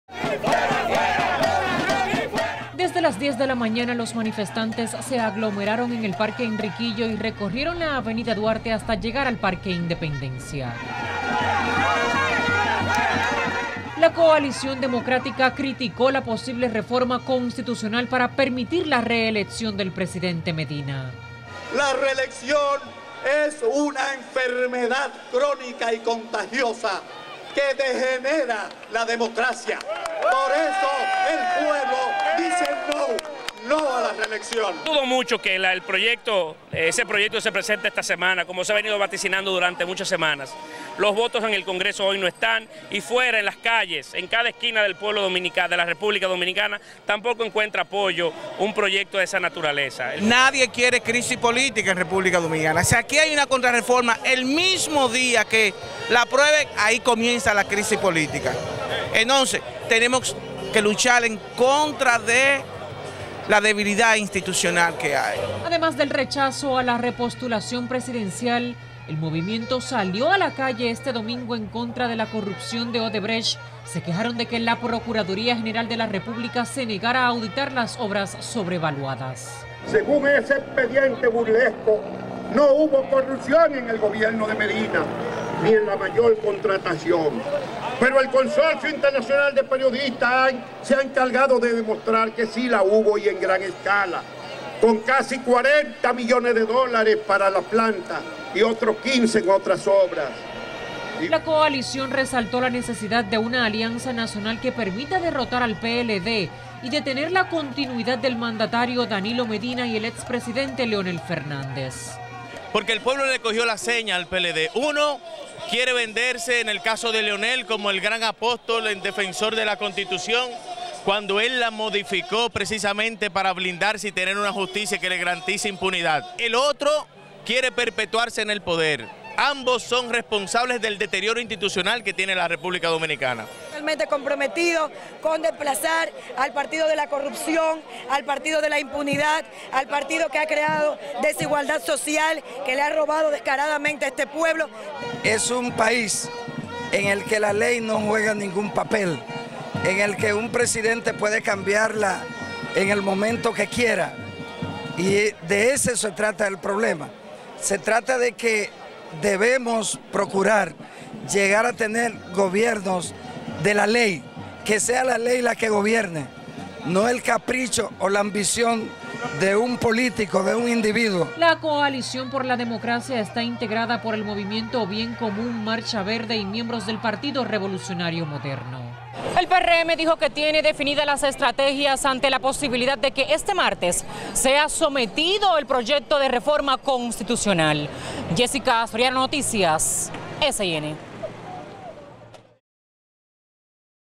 Cientos de militantes del PRM y grupos sociales marchan en contra de la reelección
SANTO DOMINGO.- Cientos de militantes del PRM, grupos sociales y otros partidos políticos, marcharon este domingo contra la corrupción, la impunidad y el secuestro institucional, en una actividad convocada por la Coalición Democrática.

La manifestación también fue contra una posible reforma constitucional que permita la repostulación del presidente Medina.